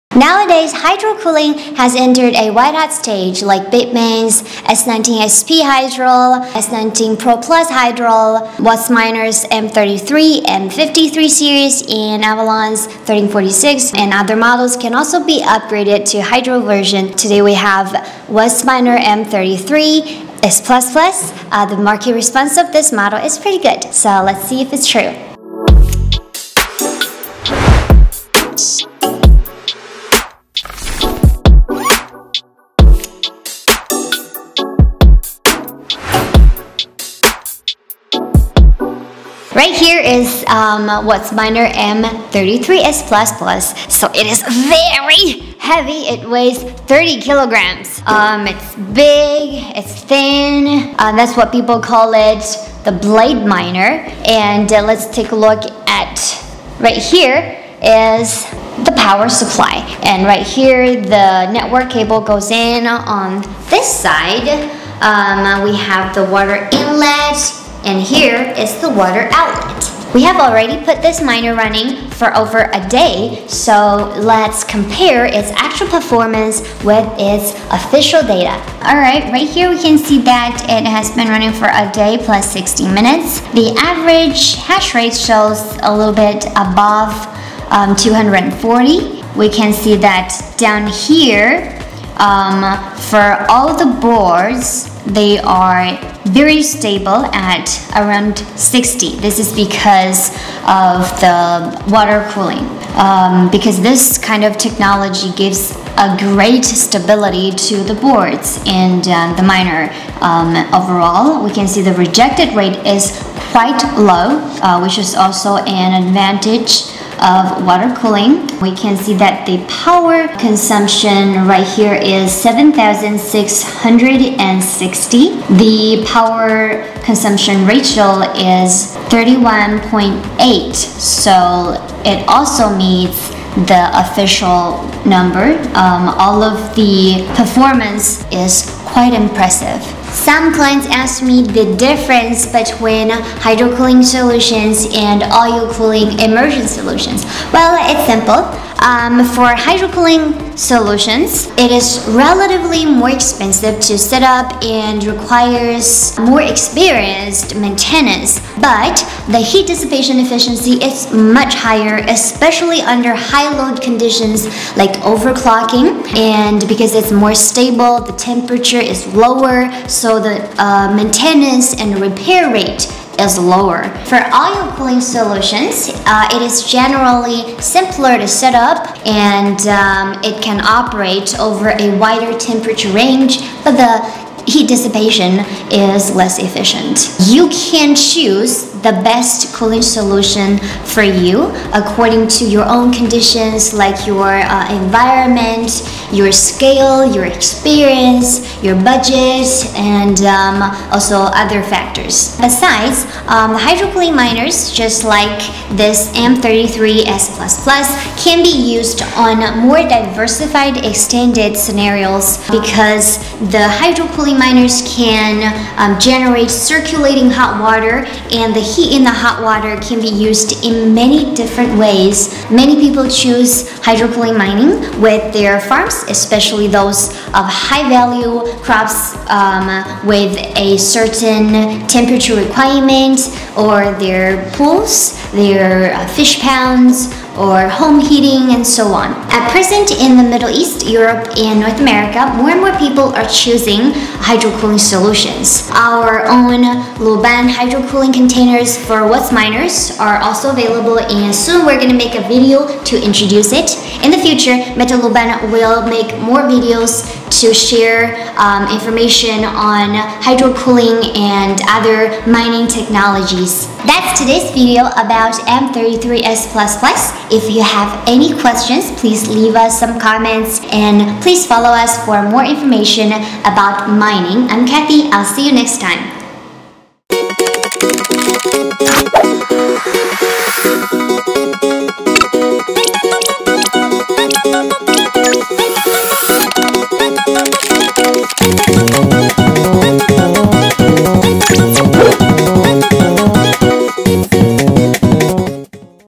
WhatsMiner hydro cooling M33/M53 Review（神马M33S++/M53测评）
Now hydro-cooling mining has entered a white-hot stage, like BITMAIN's S19 XP hydro, S19 pro+ hydro,and Whatsminer's M33/M53 series, Avalon A1346 and other models can also be upgraded to hydro cooling.

Today we received the Whatsmart M33S++. The market response of this water-cooled model is still good, let’s take a look.

Official Introduction.

Business Cooperation.